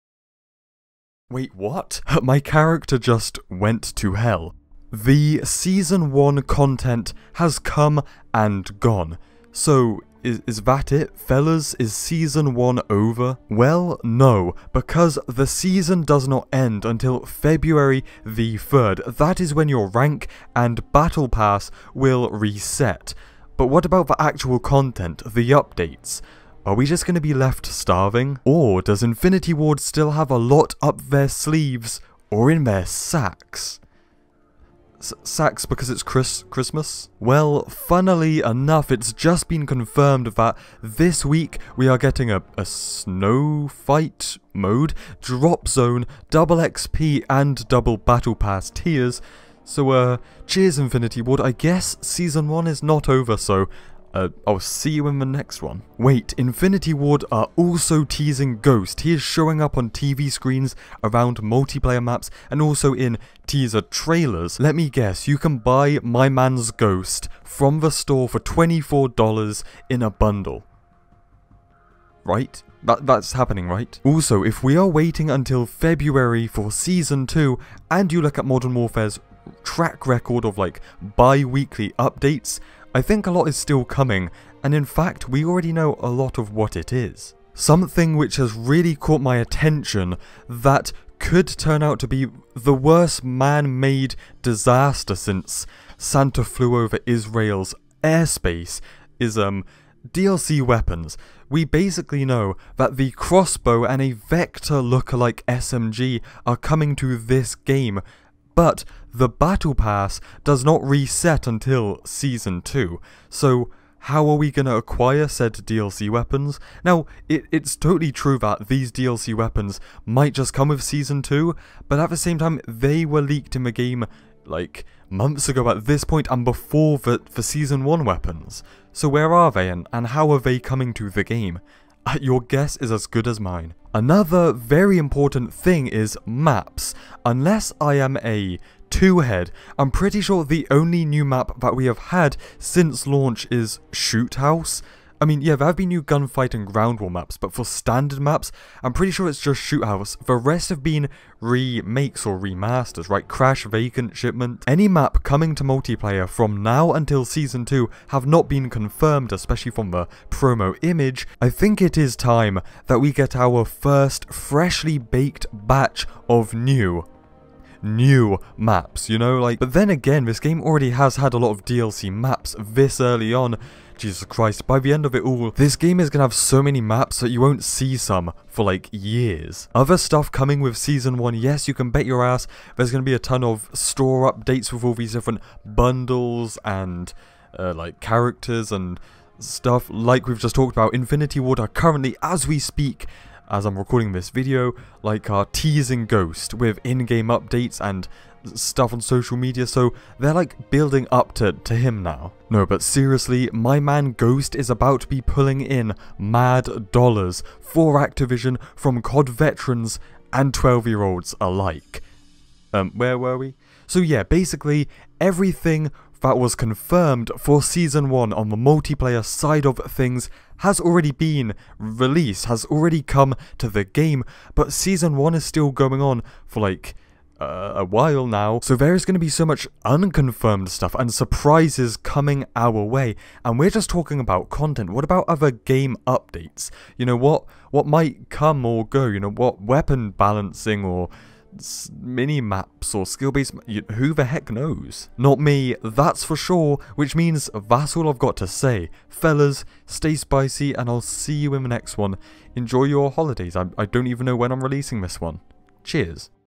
Call of Duty Modern Warfare Season 1 More Updates? - COD MW 2019
Call of Duty Modern Warfare Season 1 lasts until February the 3rd, so what else does Infinity Ward have in store for COD MW? unconfirmed New Maps, modes, updates & more.
Remember to Like & Sub :D